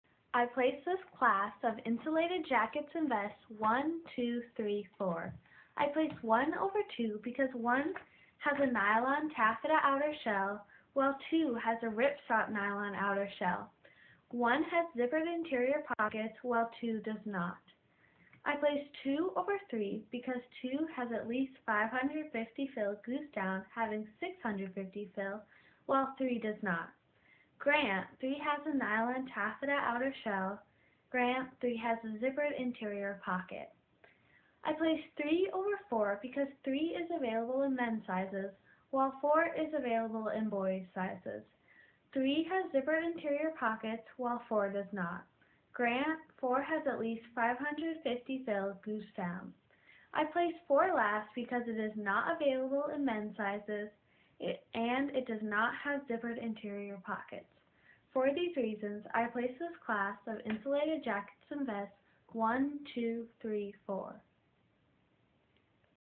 4-H Consumer Decision Making Oral Reasons Example
4-H youth defend their class placings by presenting oral reasons to a judge. Oral reasons explain the order in which a youth placed items in a class and compare the differences among the items.